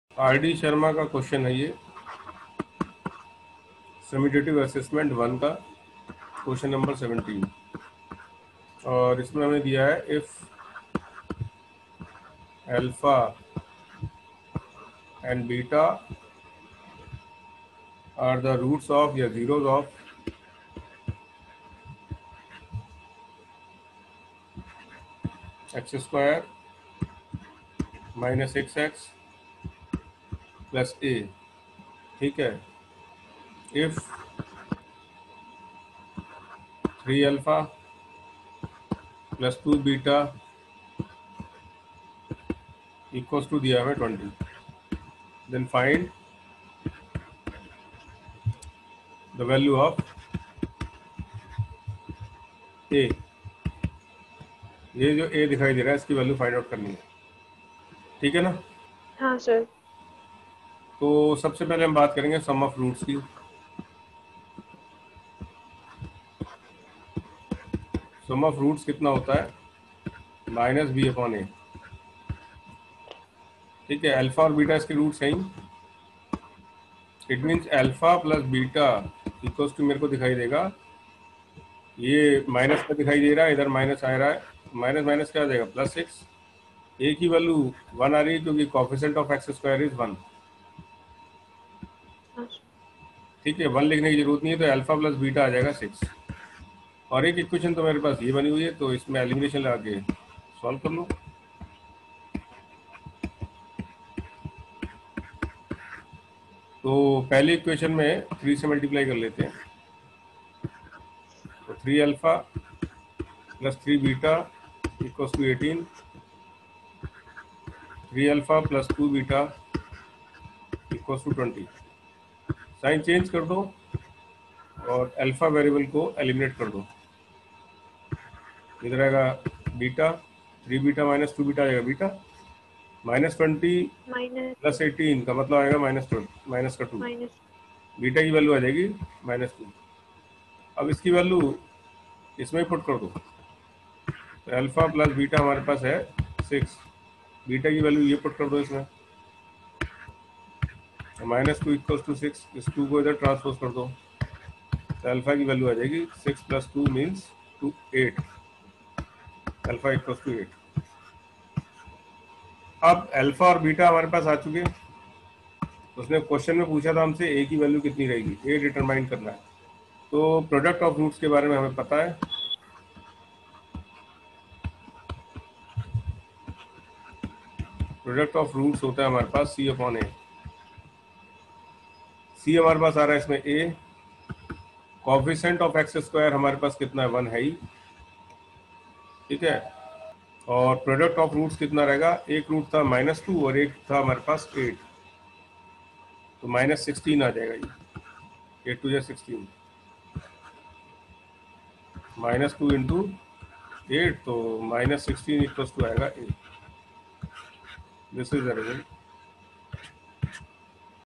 SUMMATIVE ASSESSMENT 1 QUESTION 17 POLYNOMIALS R D SHARMA
In this video, I have solved question 17 summative assessment  1 R D  SHARMA. Question related to polynomials.